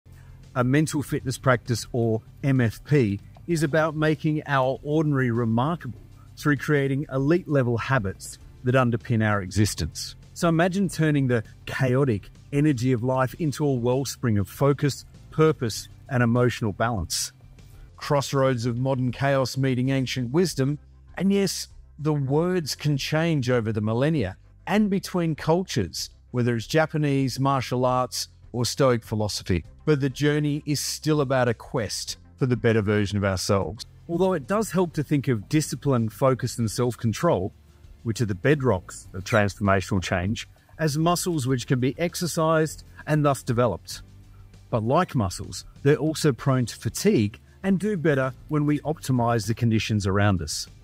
Dodge Life's Curveballs Like a Pro: Mental Fitness Magic
Life throws curveballs left, right, and center, but with a baseball bat called “Mental Fitness,” we can smack the balls out of the park, Take setbacks like a pro, and turn our mental struggles into mental strengths.

Let's team up and tackle those mental hurdles head-on.
Launch the scorecard today and Enrol now for some mental fitness magic!

Once you have completed the 2-minute “Life Potential Scorecard” you can book yourself in for a FREE (and obligation-free) 30-minute coaching session to discuss your results.